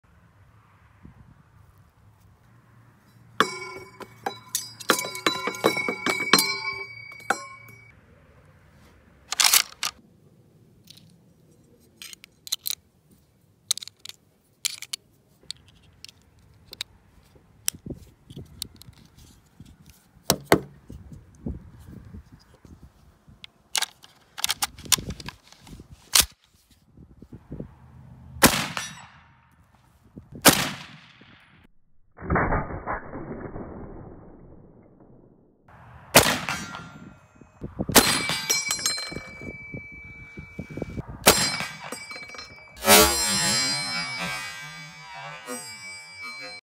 M1 Garand
Shooting my 1944 M1 Garand service grade.